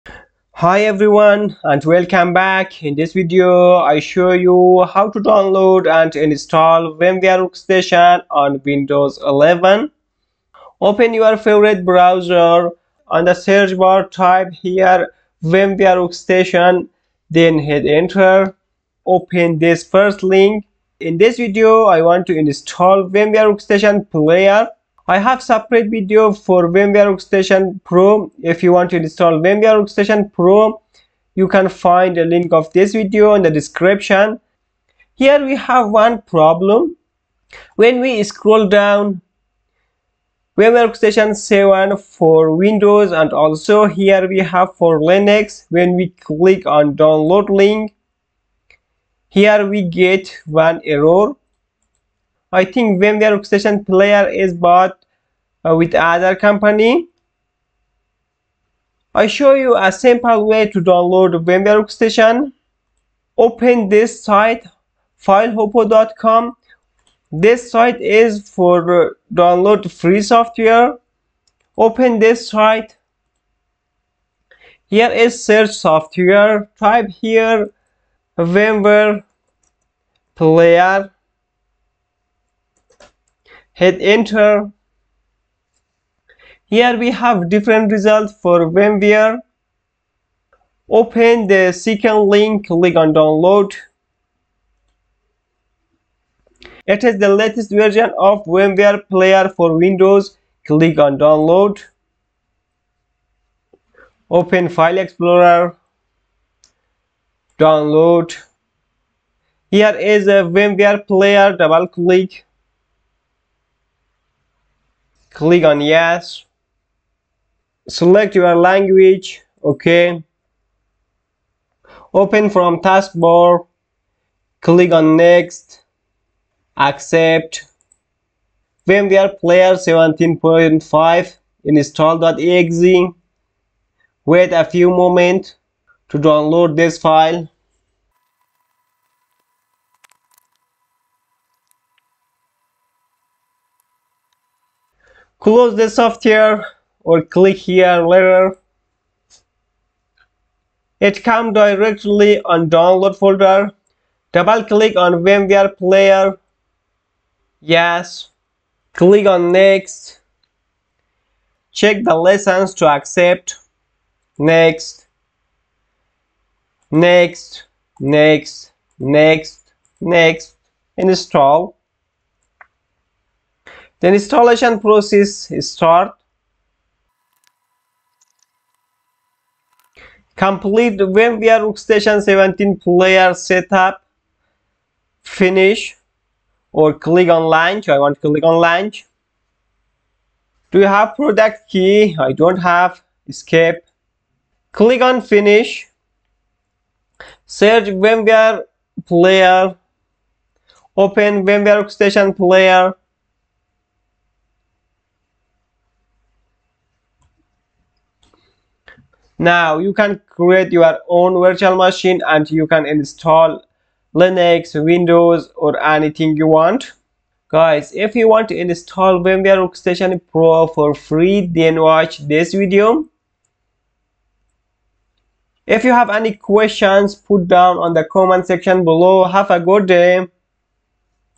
How to Install VMware Workstation 17 on Windows 11
In this video, I'll guide you through the step-by-step process of installing VMware Workstation 17 on a Windows 11 system. Whether you're a beginner or an experienced user, this tutorial will help you set up your virtual environment with ease. From downloading the installer to configuring the virtual machine settings, we've got you covered.

✅Key Points:✅
  This video tutorial covers the essential steps to download, install, and configure VMware Workstation 17 on Windows 11. It starts with navigating the official VMware website to obtain the correct installer

✅ Benefits:✅
  By following this tutorial, you'll gain the skills needed to efficiently set up VMware Workstation 17, enabling you to run multiple operating systems simultaneously on your Windows 11 machine. This is particularly beneficial for software testing, development, and learning purposes, as it allows you to create isolated environments without impacting your primary OS. Additionally, mastering VMware Workstation enhances your technical expertise, making you more versatile in handling different operating systems and applications.